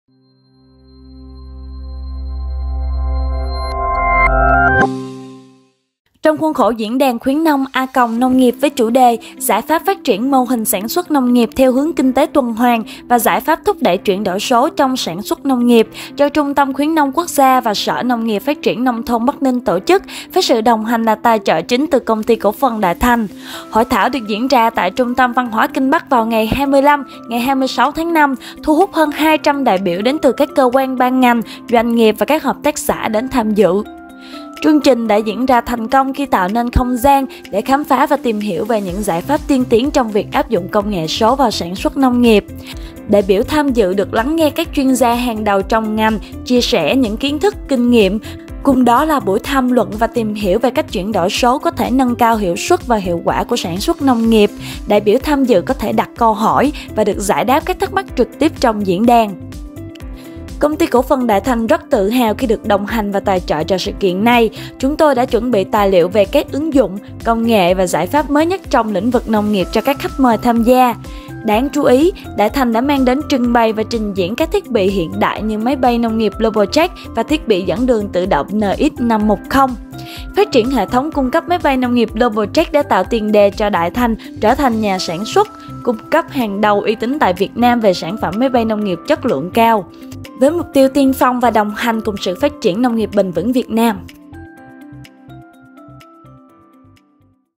Phát triển mô hình sản xuất theo hướng kinh tế tuần hoàn và thúc đẩy chuyển đổi số nông nghiệp
Giải pháp phát triển mô hình sản xuất nông nghiệp theo hướng kinh tế tuần hoàn và thúc đẩy chuyển đổi số trong nông nghiệp

Trong hai ngày 25 và 26/05/2023 tại Trung tâm văn hóa Kinh Bắc, tỉnh Bắc Ninh, đã diễn ra 2 sự kiện diễn đàn là Giải pháp phát triển mô hình sản xuất nông nghiệp theo hướng kinh tế tuần hoàn và Giải pháp thúc đẩy chuyển đổi số trong sản xuất nông nghiệp. Chương trình do Trung tâm Khuyến nông quốc gia phối hợp Sở Nông nghiệp và Phát triển nông thôn tỉnh Bắc Ninh tổ chức, Công ty cổ phần Đại Thành đồng hành tài trợ.

Một trong những điểm nổi bật tại sự kiện đã thu hút sự quan tâm lớn của các vị đại biểu và khách mời chính là phần Trình diễn thiết bị dẫn đường tự động NX510 trên máy cấy. Phần trình diễn này được thực hiện bởi các nhân viên kỹ thuật của Công ty cổ phần Đại Thành thực hiện. Trình diễn thiết bị dẫn đường NX510 đã gây ấn tượng mạnh với khả năng tự động hóa quy trình cấy, giúp tăng hiệu suất và độ chính xác trong việc trồng trọt.

Ngoài trình diễn thiết bị dẫn đường tự động NX510, Đại Thành cũng đã giới thiệu đến các vị khách mời sản phẩm máy bay nông nghiệp đa chức năng Globalcheck như G100, PG40, VG40. Việc áp dụng các công nghệ số trong nông nghiệp như máy bay nông nghiệp, thiết bị dẫn đường tự động không chỉ giúp nâng cao năng suất và chất lượng sản phẩm, mà còn giảm thiểu công sức lao động, bảo vệ sức khỏe và giảm thiểu ô nhiễm môi trường.